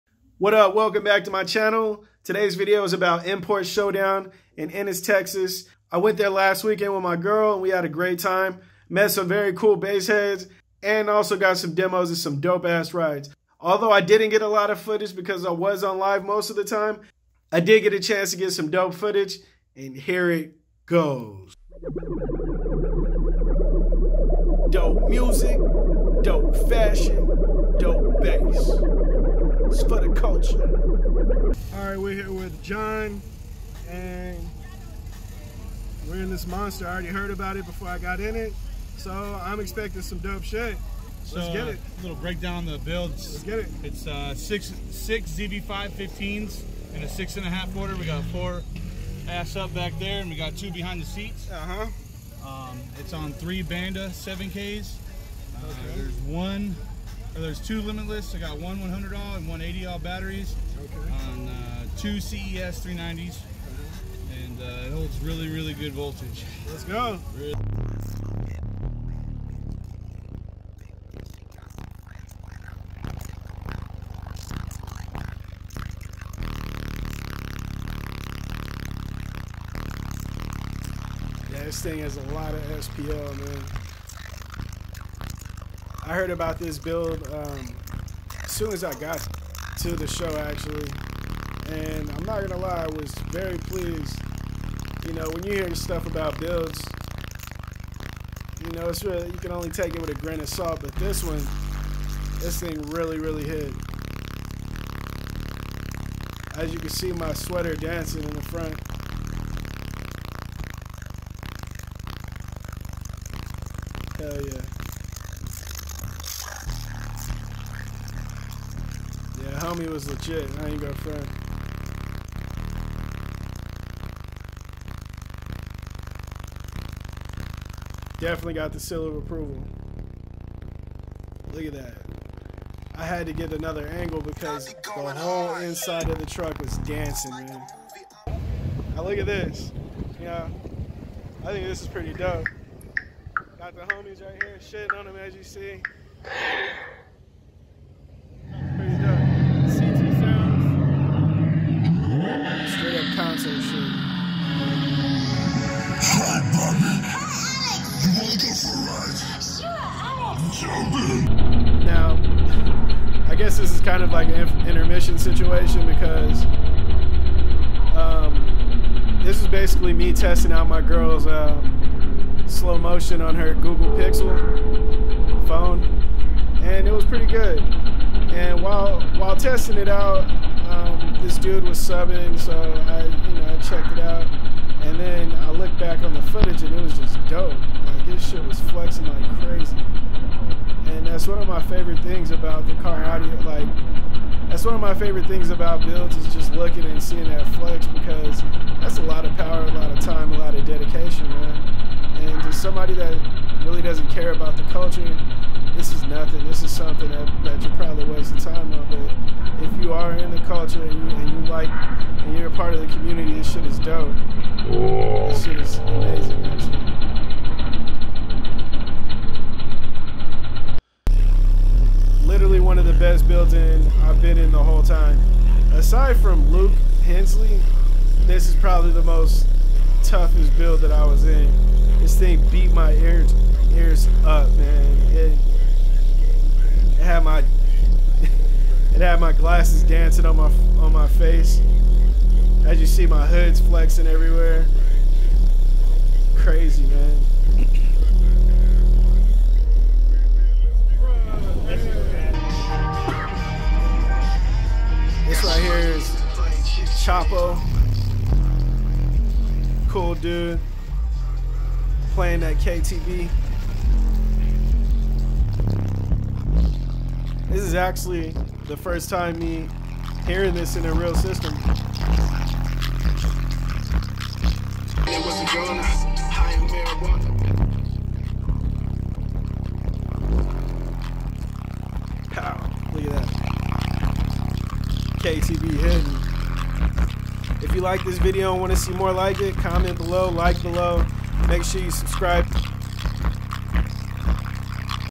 Ear Ringing Bass!! Import Showdown
This vlog takes place at Import Showdown in Ennis Texas. It was a great time. most of my footage was on live but enjoy what i got here! if you like this video and want to see more like it, comment and like!